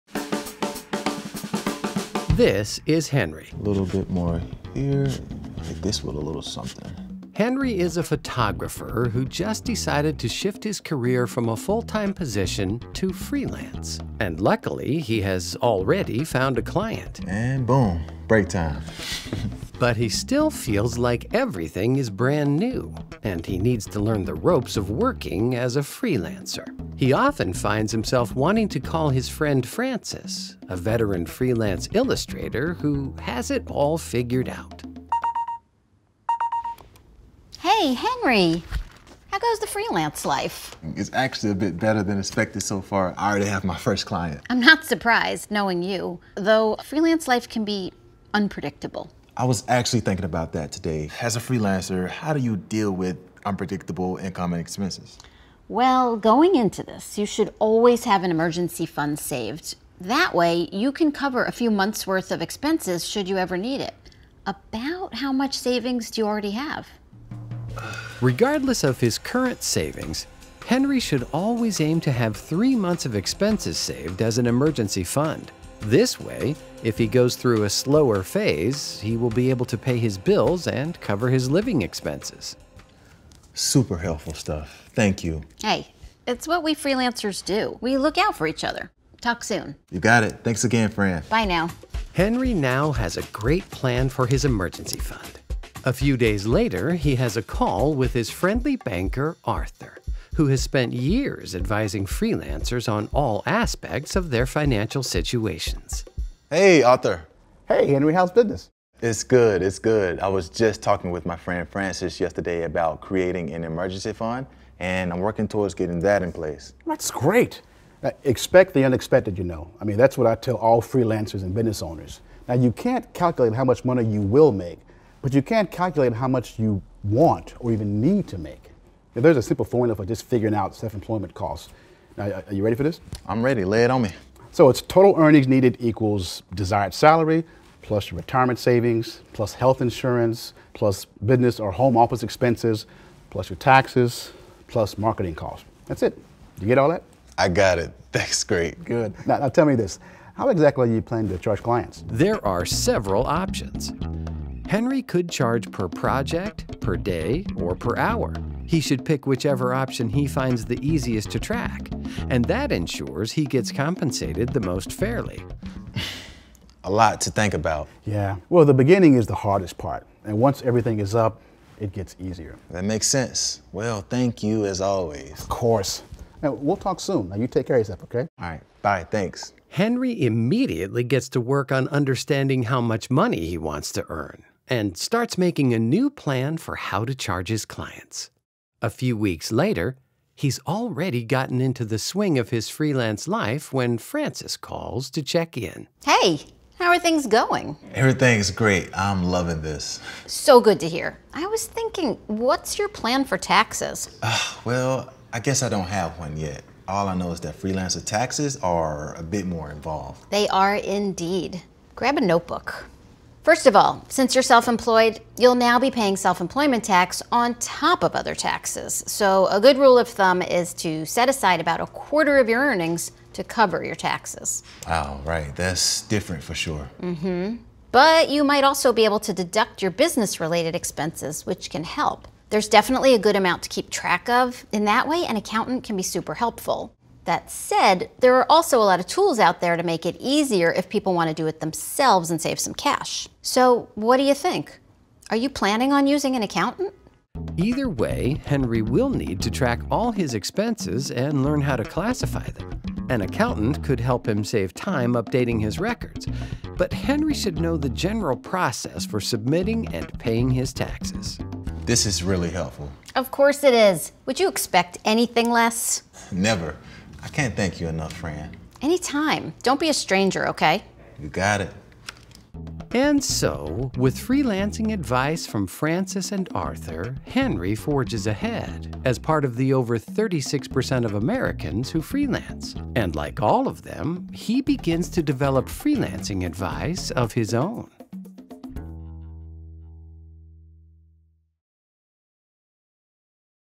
Fine-tune Your Finances: Shifting from full-time to freelance
An increasing number of Americans work for themselves. In fact, amid the covid-19 pandemic, one third of Americans work as freelancers. Switching from full-time employment to a freelance career can feel like entering a whole new world – but that doesn't mean it's impossible.